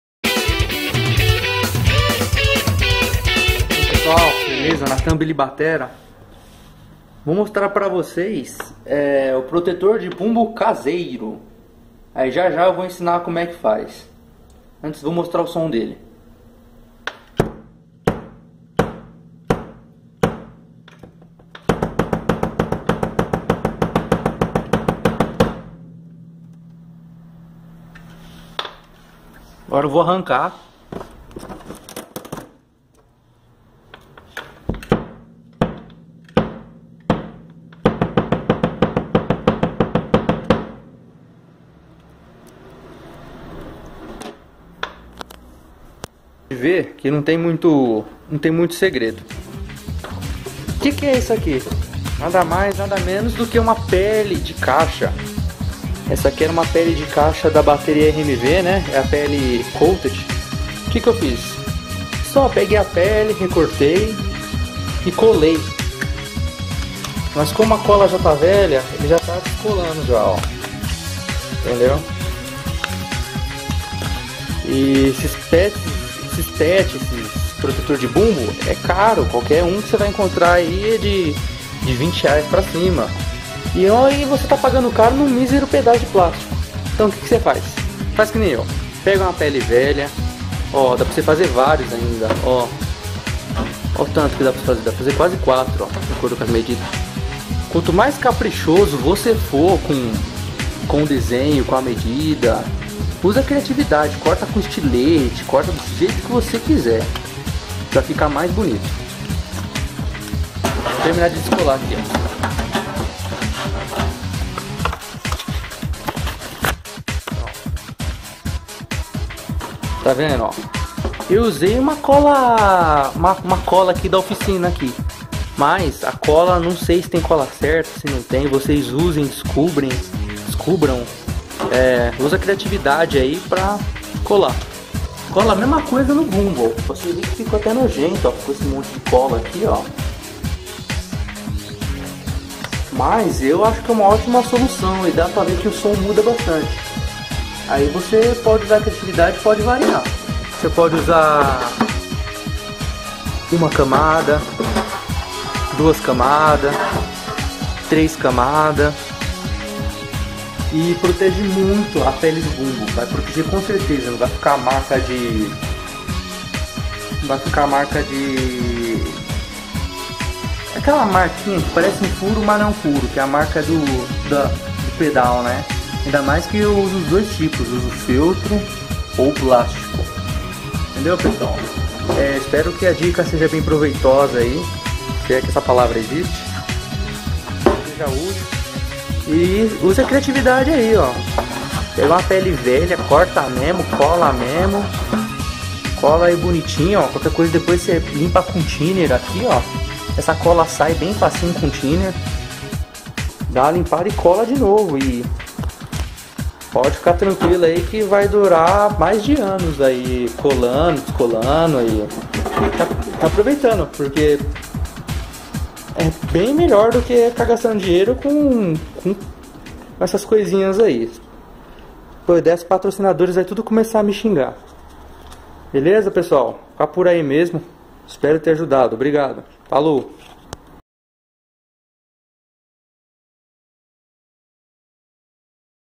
Protetor para pele de bumbo/ homemade bass drum patches
Recorte da forma como quiser,recomendo uso de estilete,e veja qual cola sera melhor para usar...talvez cola para cano de PVC sirva bem,ja que estamos tratando de plástico.

Cut out the way you want I recommend using stylus and see what glue will be better to use ... maybe glue for PVC pipe serve well since we are dealing with plastic.